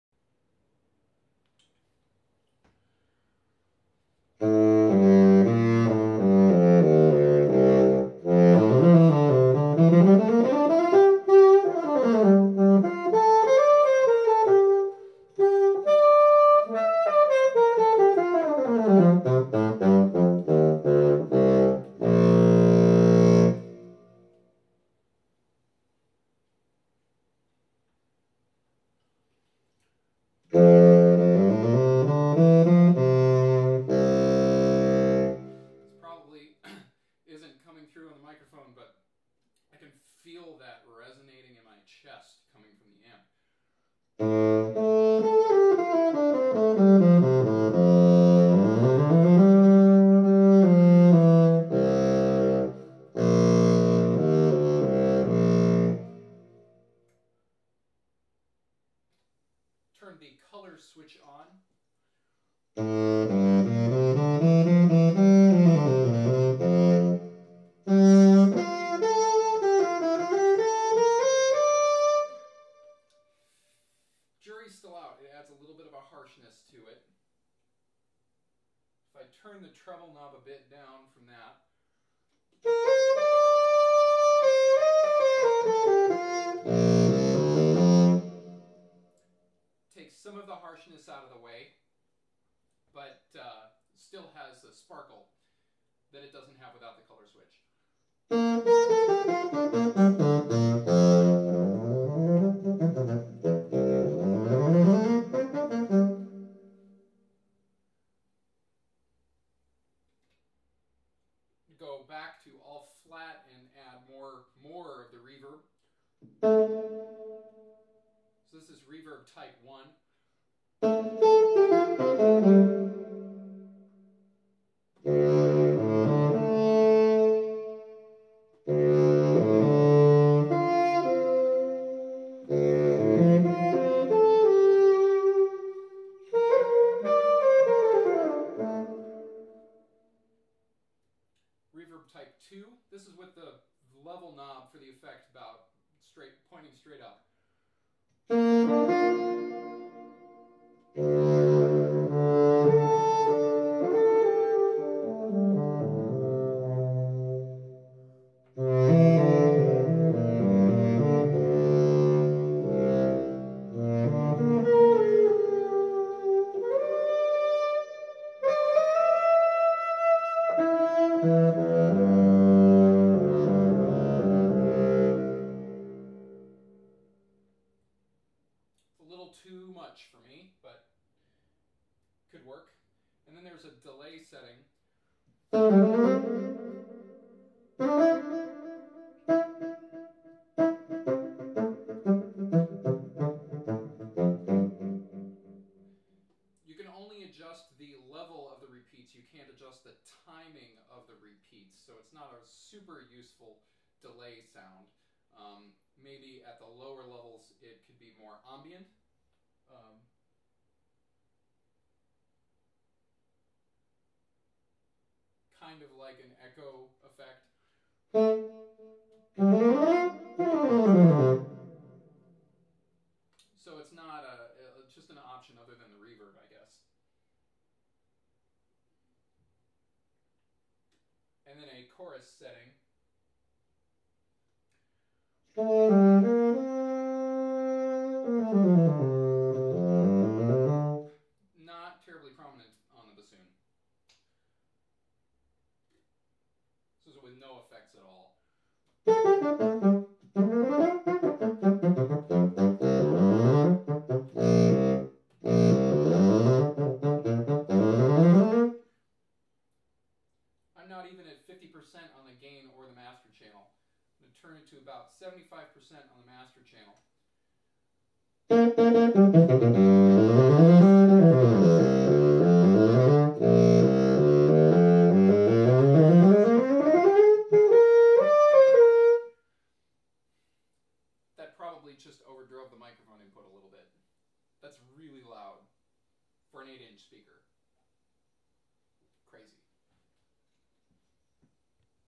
AER Compact 60/3 demo with bassoon
I received my new AER Compact 60/3 last week and have been consistently impressed with it in initial tests. The amp has a fullness and power of sound that just blows my mind for a single 8 inch speaker.

I'm running the bassoon directly to channel one of the amp with no extra effects. The reverb is from the amp reverb. I start with EQ flat and the color switch off. Master volume is about 9:00 but I turn it up part way through the video. This thing shakes the walls of my practice space. Even though the bassoon is physically in front of the amp I can tell you that you're pretty much only hearing the sound of the amp, it's just dominating in the room.

As usual I'm using my Little-Jake pickup through an L.R. Baggs preamp. Preamp EQ is set at about 11:00 on the bass and 2:00 on the treble knob, with the bass trim all the way up.